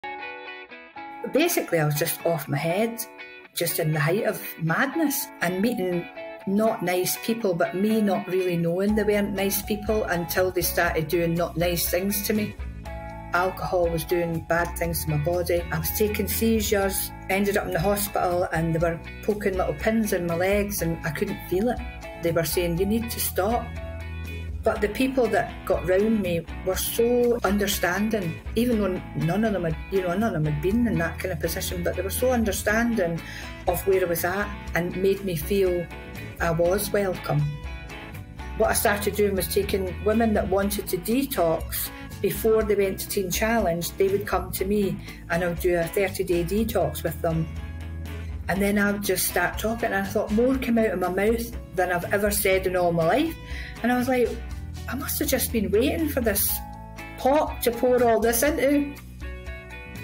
Fiona Myles – Sobering Self-reflection
Fiona Myles grew up in a small town in Scotland, always aware that she was different from her siblings because she was adopted. As a teenager, she became increasingly isolated and began to rebel. She moved to London to work as a nanny, but quickly got caught up in a world of drugs and alcohol, ending up homeless and sleeping rough in Victoria Station.

She even had a brush with death when she tried to commit suicide, but failed. She realized that she needed to make a change.

She eventually found the supportive network to help her rebuild her life and began helping other girls in similar situations.

While looking after her own adopted daughter, she began to write her story, finding that the words flowed out, and led her to a more positive, empowered life.